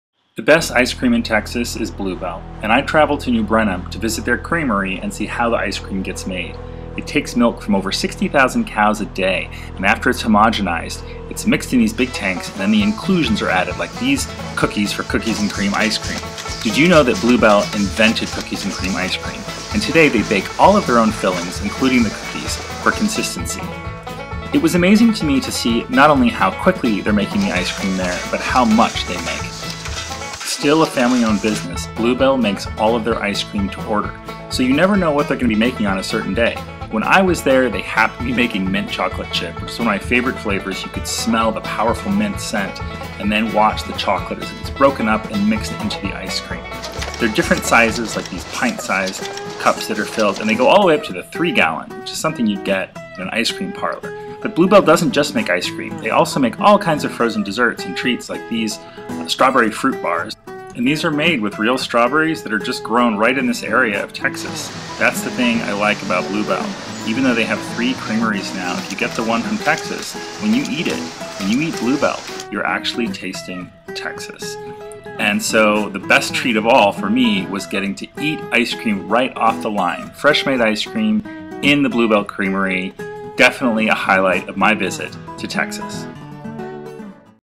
Inside the Blue Bell Ice Cream Factory
Visiting Blue Bell Creamery in Brenham, Texas--the best ice cream in America! Such a fun tour, I would advise anyone in and around Houston to go visit. Must tries: Lemon Bliss, Red Velvet and Moo-lennium Crunch!